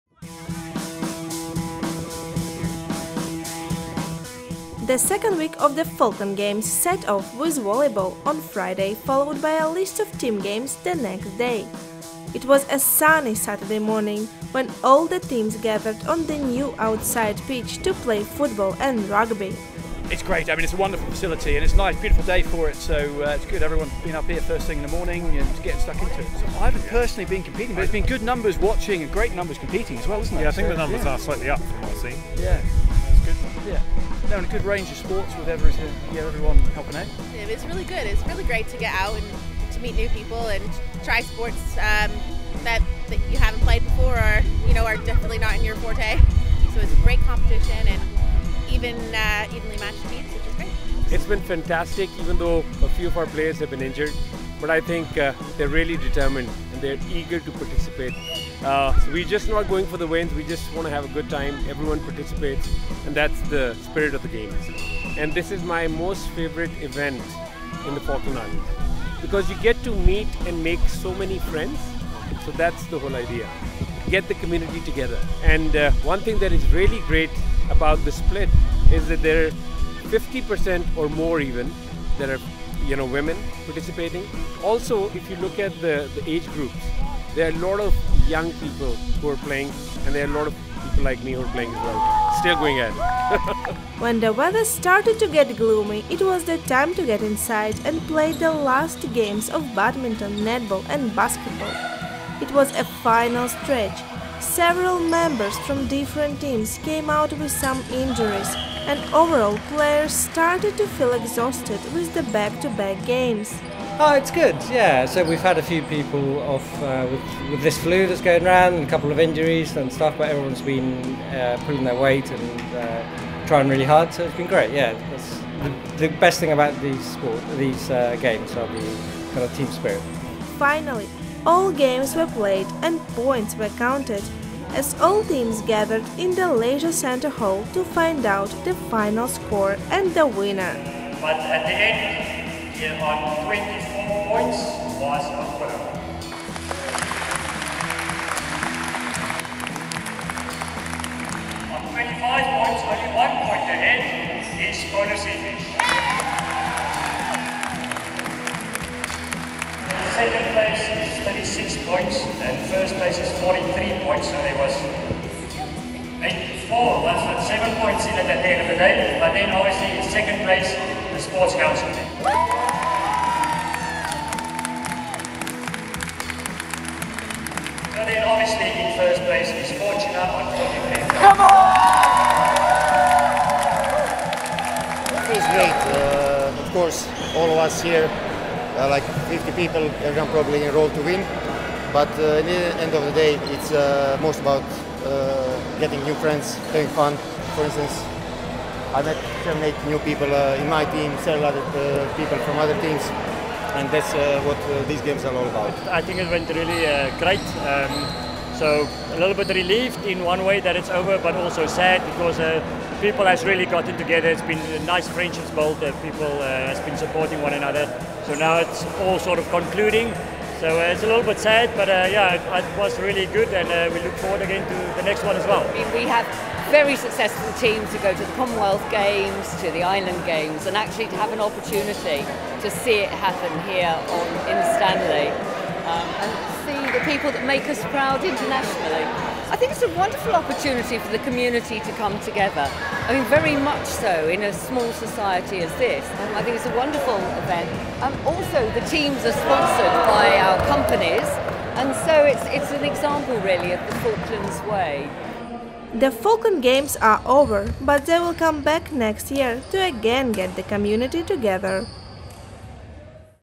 The Falkland Games second week's action
The Falkland Games have come to an end for another year. It’s been exciting two weekends for both players and spectators. And the long-awaited award ceremony was held last Saturday. So keep watching if you want to know who won the Falkland Games.